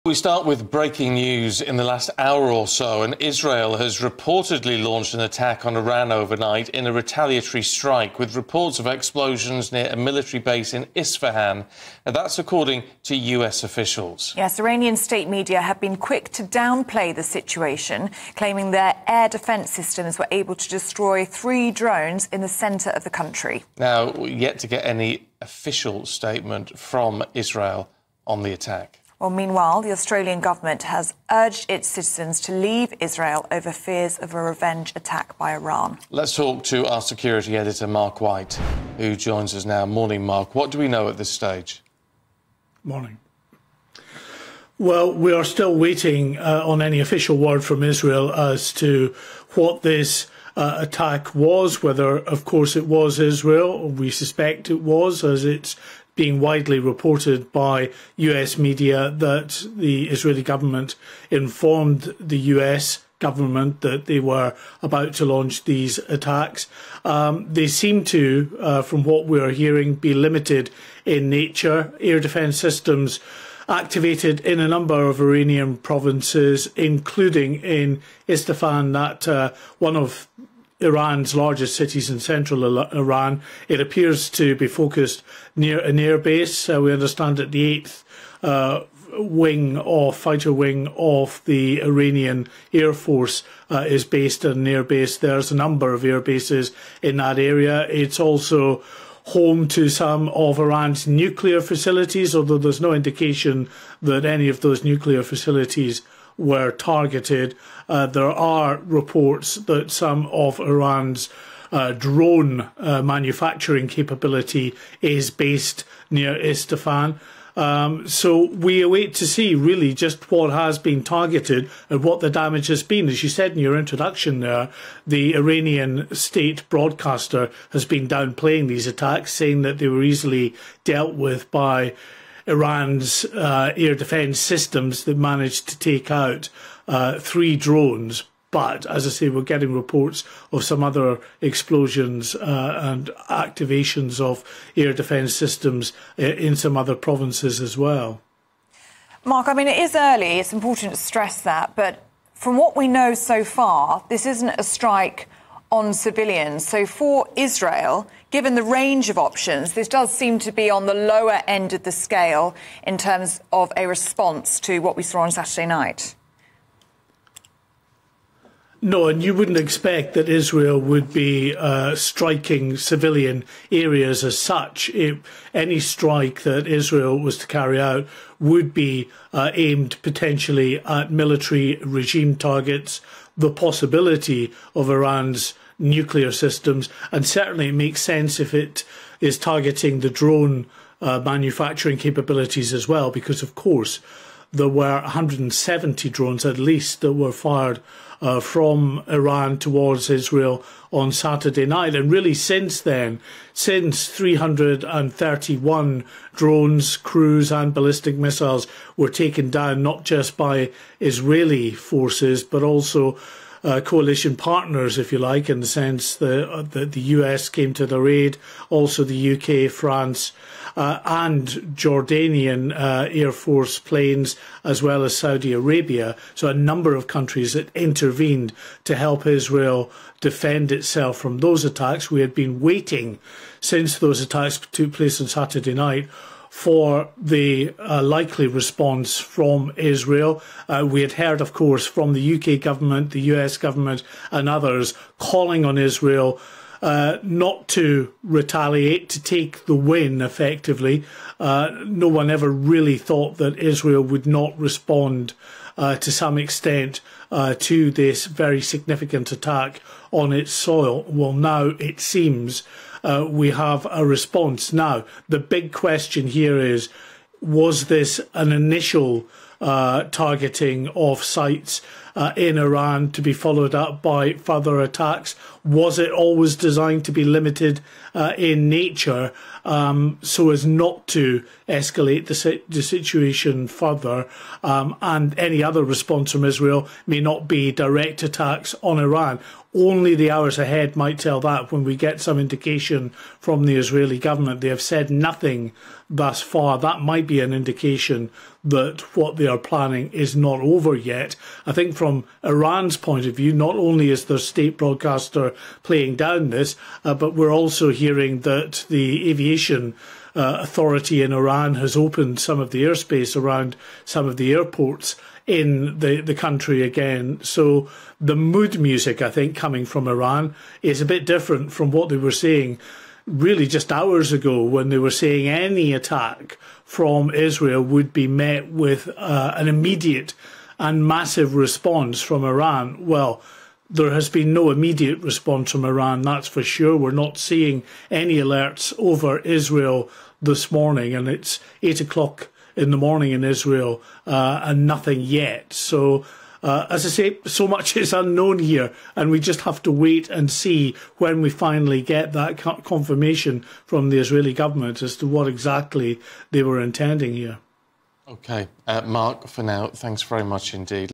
Israel attacks Iran: Middle East on edge after explosions heard in Isfahan | Latest with Mark White
BREAKING: American officials claim that Israel have launched an attack on Iran.

Mark White provides the latest on the escalating conflict in the middle east.